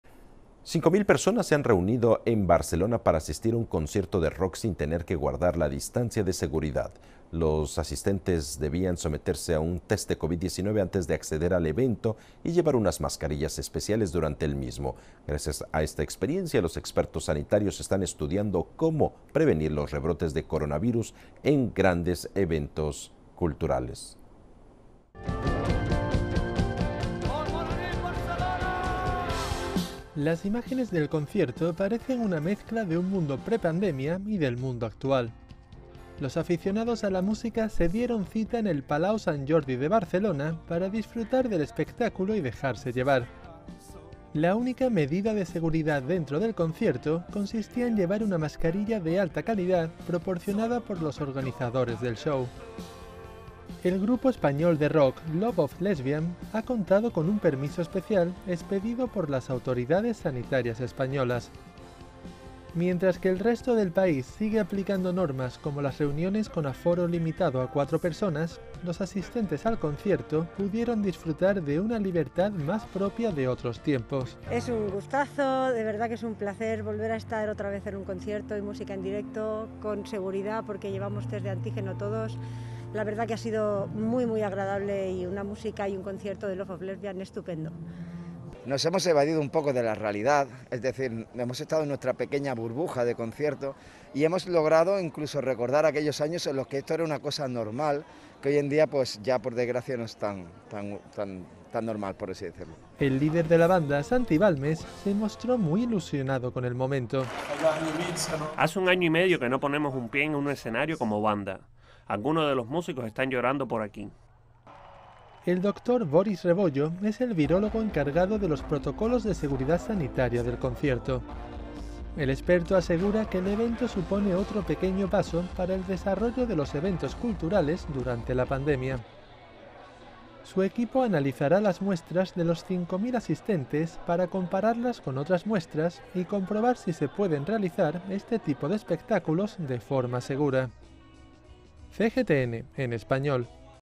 5.000 personas asisten a un concierto en Barcelona
5.000 personas se han reunido en Barcelona para asistir a un concierto de rock sin tener que guardar la distancia de seguridad. Los asistentes debían someterse a un test de COVID-19 antes de acceder al evento y llevar unas mascarillas especiales durante el mismo. Gracias a esta experiencia, los expertos sanitarios están estudiando cómo prevenir los rebrotes de coronavirus en grandes eventos culturales.